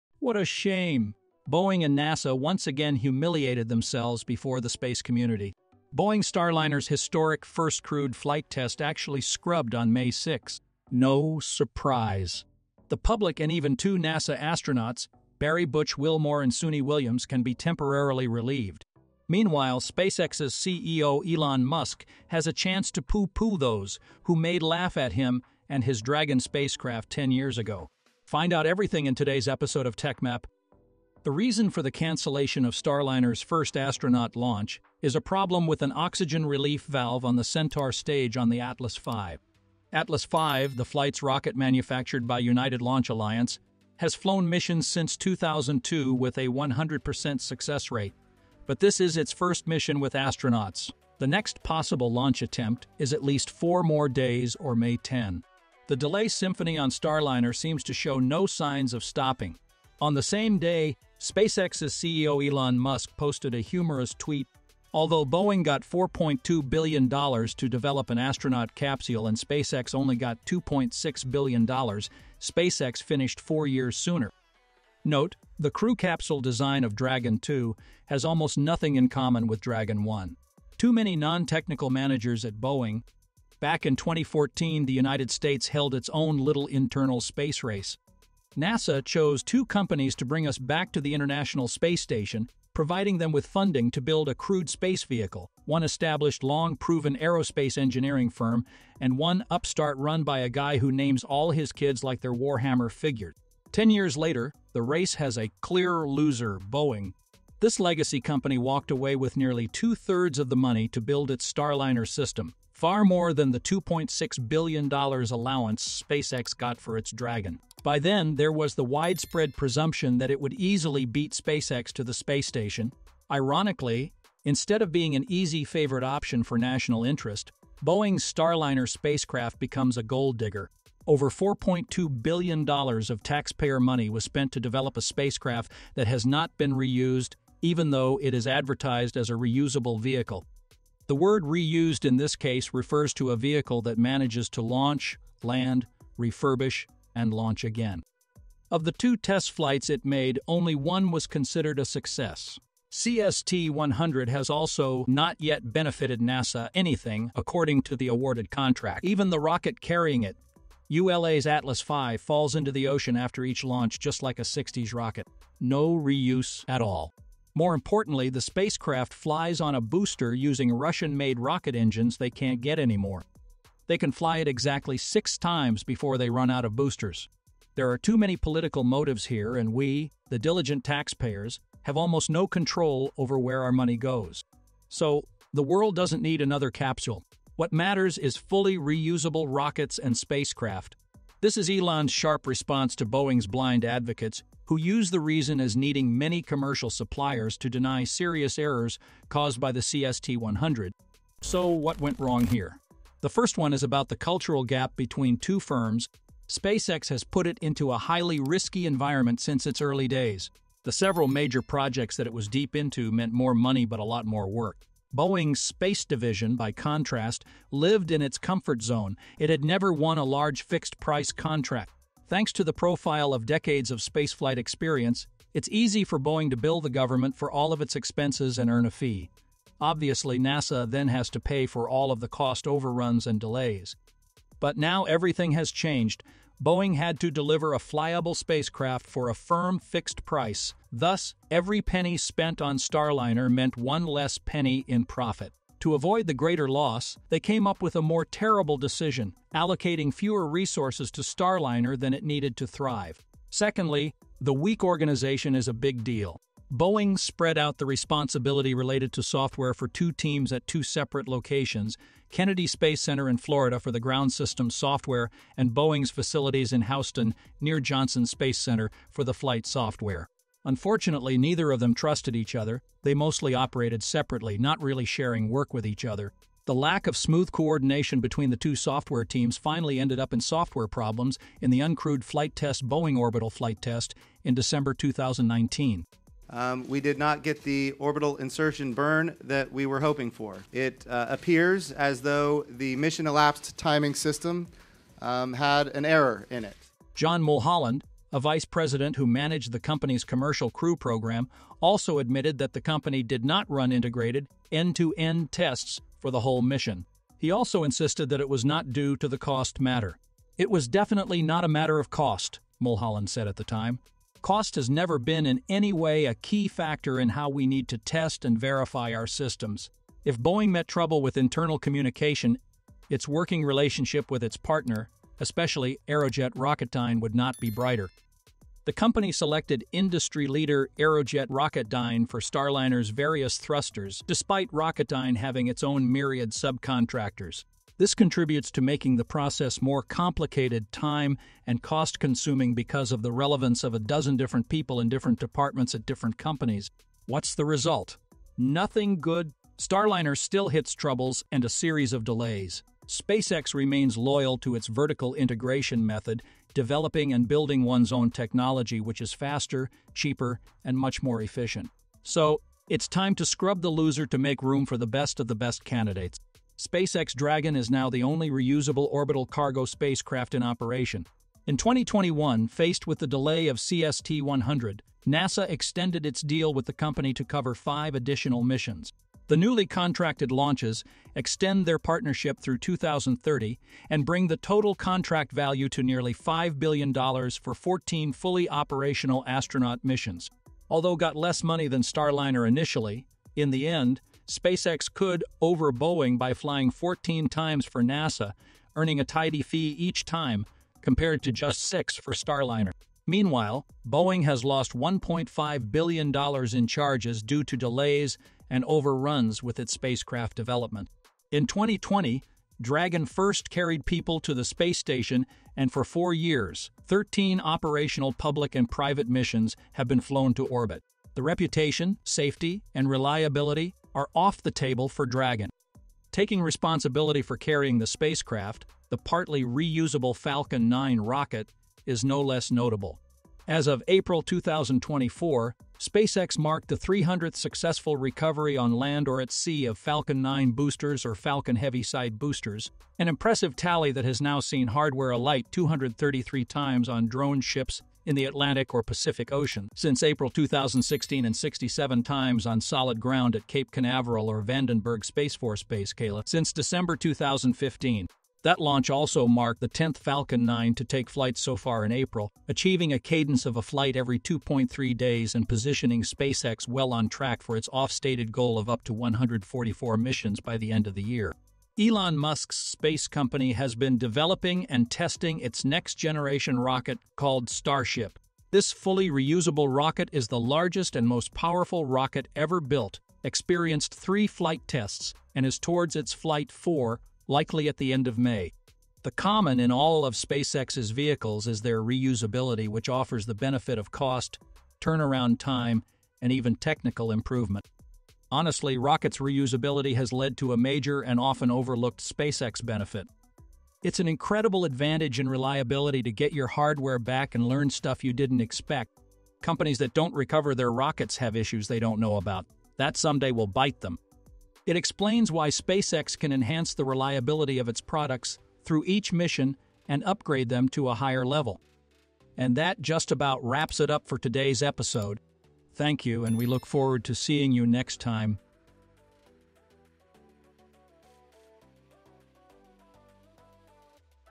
NASA Made BIG Mistake With Boeing Starliner... Musk Laugh!
(Intro) 0:00
(Starliner’s delay) 0:31
(Why did Starliner fail?) 3:21
(Outro) 8:42
Boeing and Nasa once again humiliated themselves before the space community.
Boeing Starliner's historic first crewed flight test actually scrubbed on May 6. No surprise!
The public and even two Nasa astronauts: Barry "Butch" Wilmore and Suni Williams can be temporarily relieved.
Meanwhile, SpaceX's CEO, Elon Musk has a chance to poo-poo those who made laugh at him and his Dragon spacecraft ten years ago.
The reason for the cancellation of Starliner's first astronaut launch is a problem with an oxygen relief valve on the Centaur Stage on the Atlas 5. Atlas 5, the flight's rocket manufactured by United Launch Alliance, has flown missions since 2002 with a 100 percent success rate, but this is its first mission with astronauts. The next possible launch attempt is at least four more days or May 10.
The "delay" symphony on Starliner seems to show no signs of stopping. On the same day, SpaceX's CEO, Elon Musk posted a humourous tweet:
"Although Boeing got 4.2 billion dollars to develop an astronaut capsule and SpaceX only got 2.6 billion dollars, SpaceX finished 4 years sooner.
Note, the crew capsule design of Dragon 2 has almost nothing in common with Dragon 1.